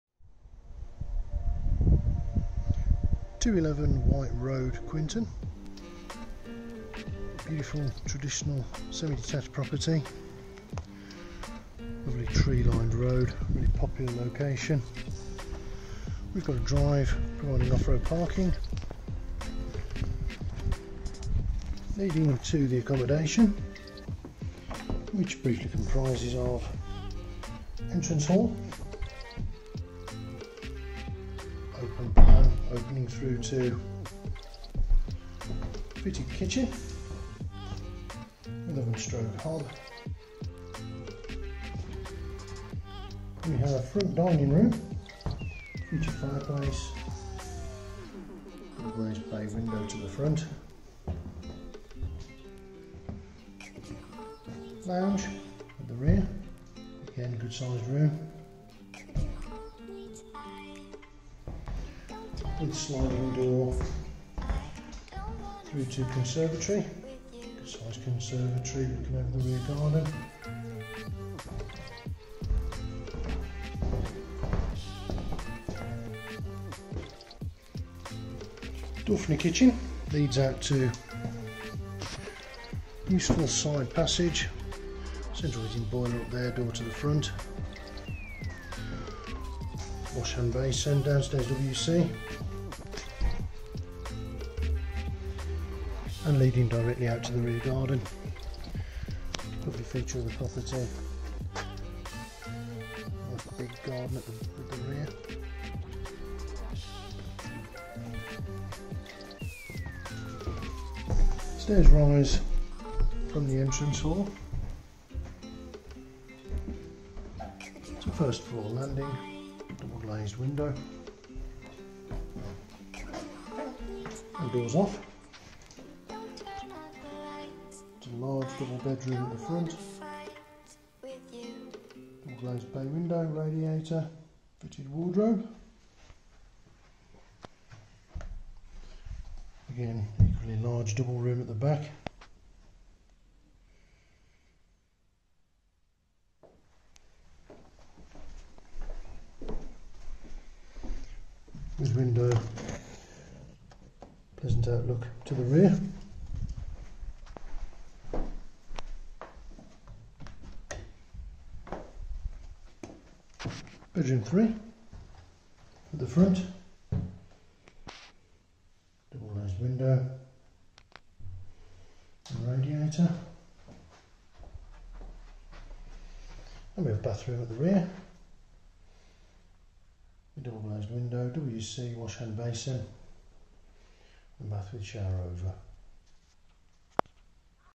White Road, Quinton (Sold STC)
Offered with NO UPWARD CHAIN, This is a great opportunity to purchase a good sized semi detached home within a very popular road in the Quinton Area, offering further potential to extend (subject to relevant planning permission). Set back behind a drive providing off road parking and leading to the following accommodation :- On the ground floor is an entrance hall opening through to Fitted kitchen, dining room, lounge and conservatory. First floor provides 3 bedrooms and bathroom and outside is a large rear garden. Double glazed and Gas central heating. EPC rating D.